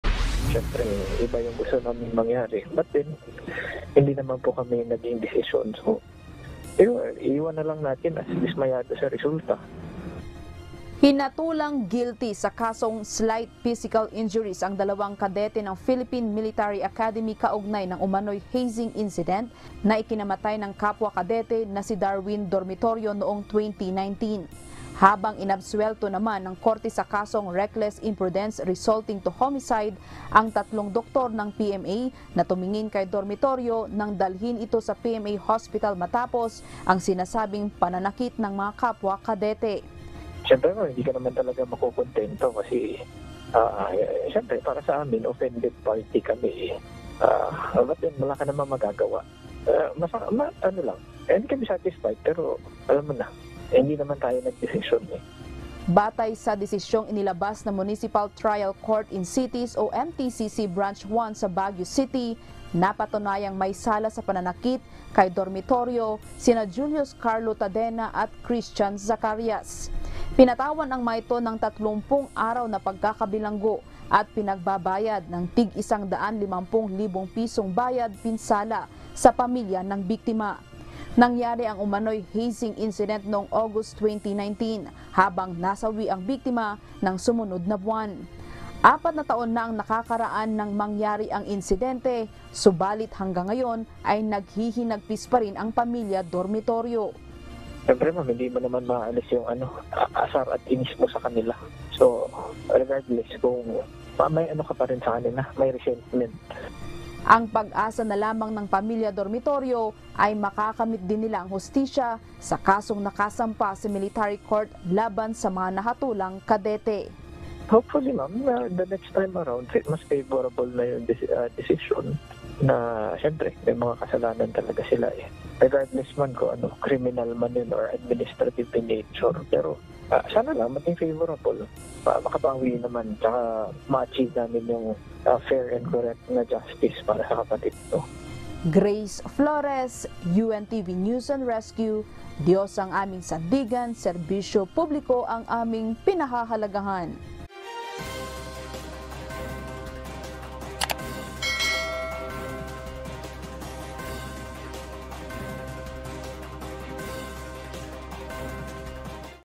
Pamilya ng nasawing PMA cadet na si Darwin Dormitorio, ‘di kuntento sa hatol ng korte
Hindi kuntento ang pamilya ni PMA cadet Darwin Dormitorio sa naging hatol ng korte sa mga akusado sa pagkamatay nito dahil sa umano’y hazing incident.

Hinatulang guilty ng korte ang dalawa sa mga akusado at sinentensyahan ng isang buwang pagkakakulong.